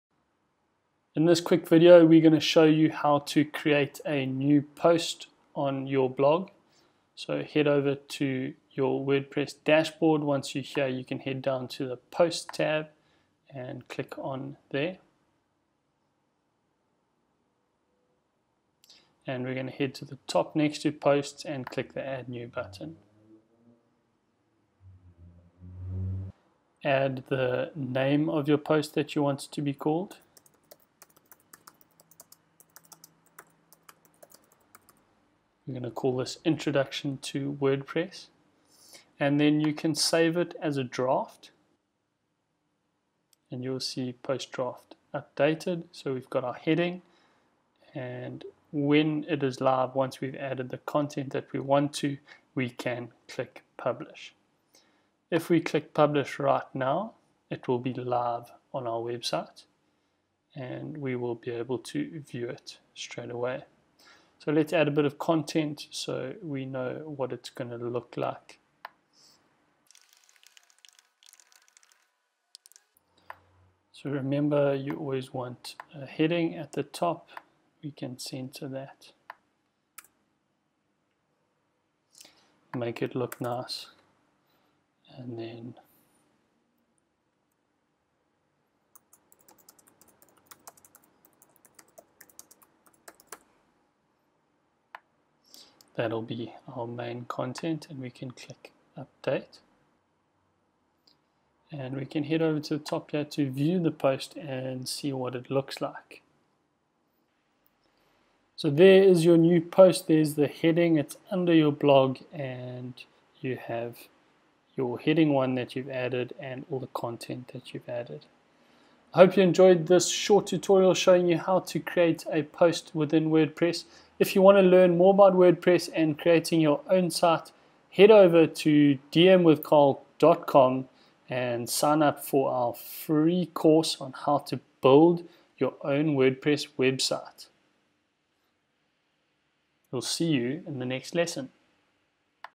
How to Create a New Post in Wordpress
This Video will show you how to create a new post in Wordpress. If you are starting a blog and want to learn how to create a new post for your blog this tutorial will help you.

If you want to learn more about Wordpress check out our Wordpress Course